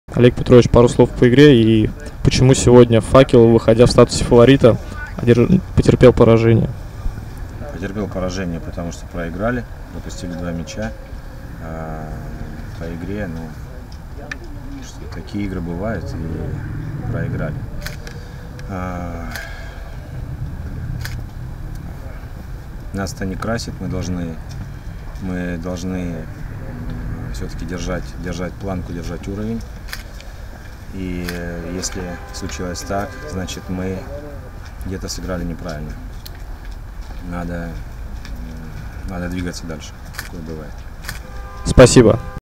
09.10.2021 Послематчевый комментарий Олега Василенко
Выступление тренера на пресс-конференции после поражения в матче «Олимп-Долгопрудный» - «Факел» со счетом 0:2.